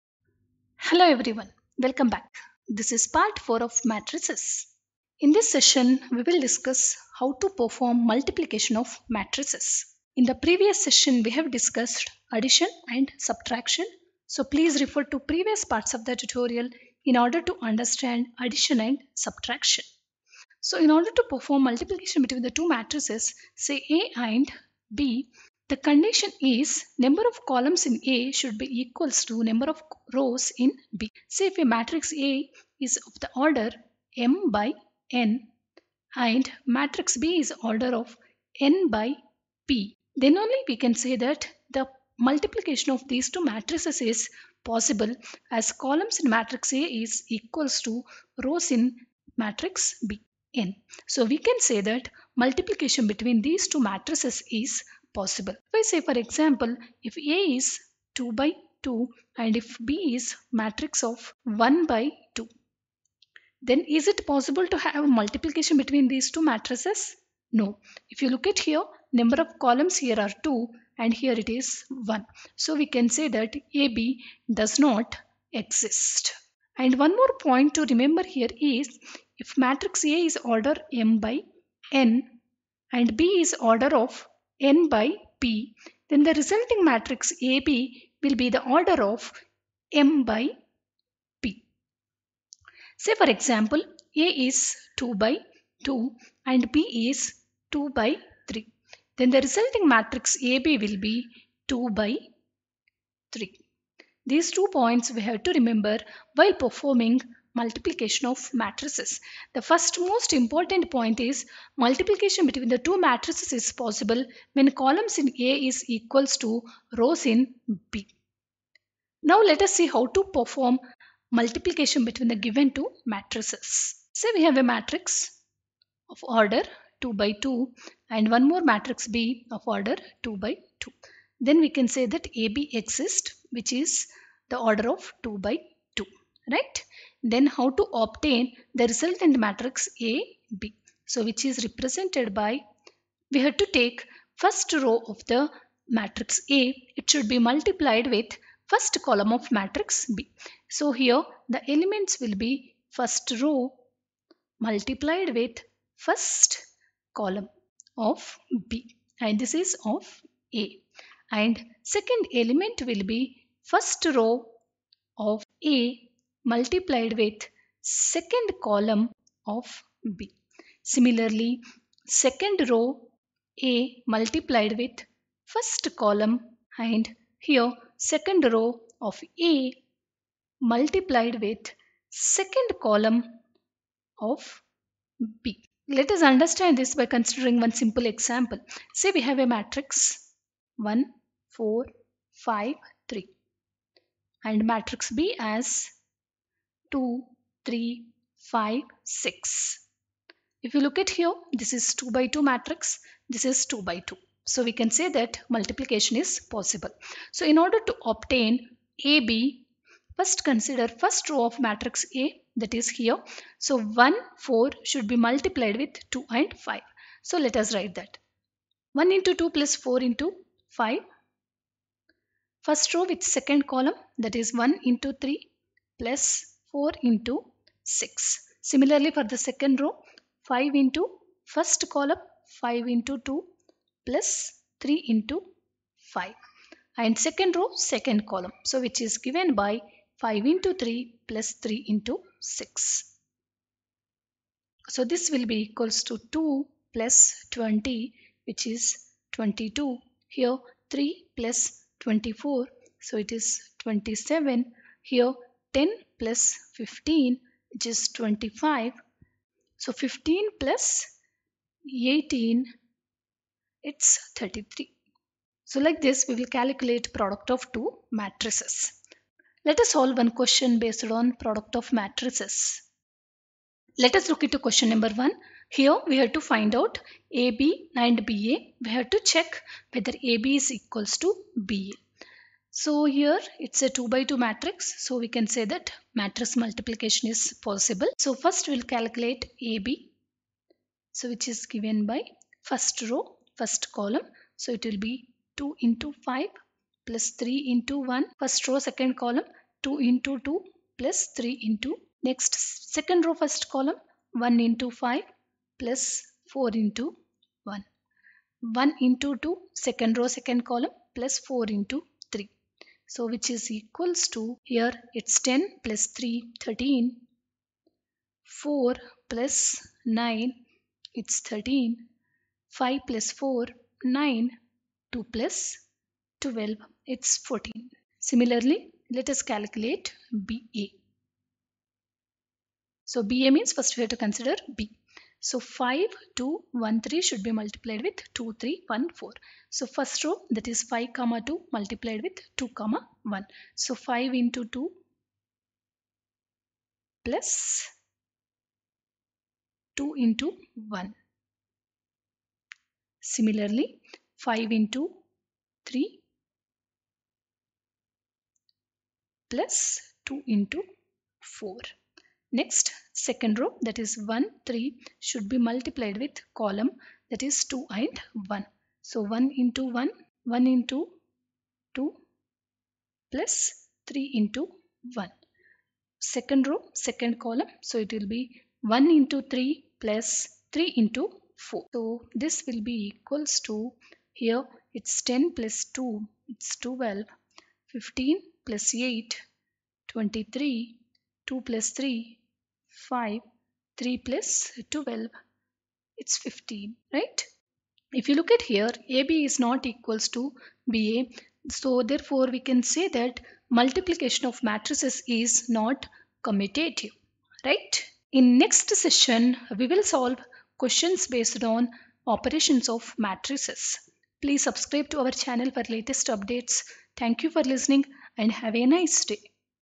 Matrix Multiplication || Multiplying Matrices|Class 10 ICSE Maths
Matrix multiplication : In order to perform multiplication between the matrices the number of columns of first matrix should be equal number of rows in second matrix.

E.g   If Matrices  A and B are given then product matrix AB exists only if

Number of columns of Matrix A= Number of rows of Matrix B

Product matrix is obtained by multiplying elements in row  with

corresponding elements in column  of matrix B and adding the

result.

If matrix A is order m by n and Matrix B of order  n by p  then

product matrix order AB will be m by p.

Matrix multiplication does not satisfy commutative law.